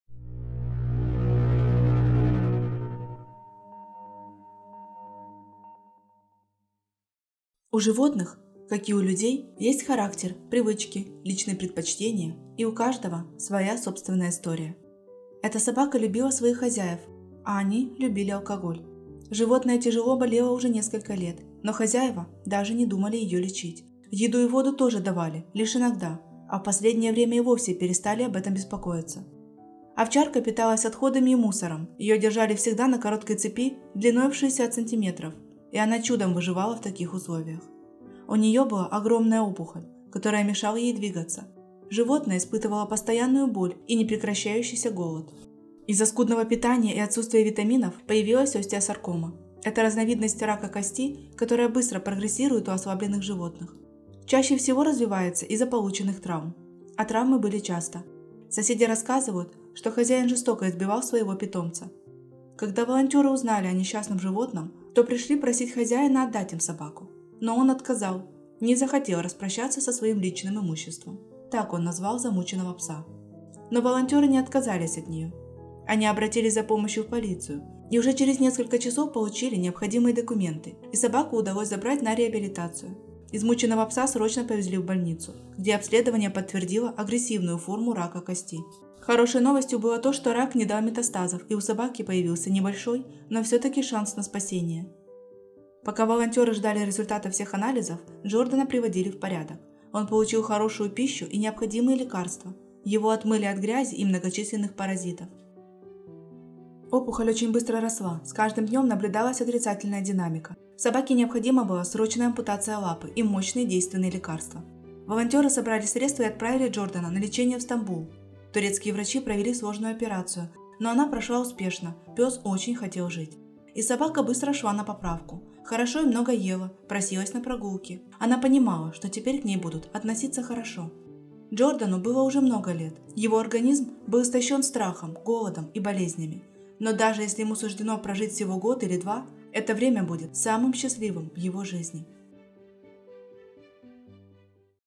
Больную собаку с опухолью мучили много лет. История спасения Джордана
Ее держали всегда на короткой цепи длиною в 60 сантиметров и она чудом выживала в таких условиях. У нее была огромная опухоль, которая мешала ей двигаться. Животное испытывало постоянную боль и непрекращающийся голод.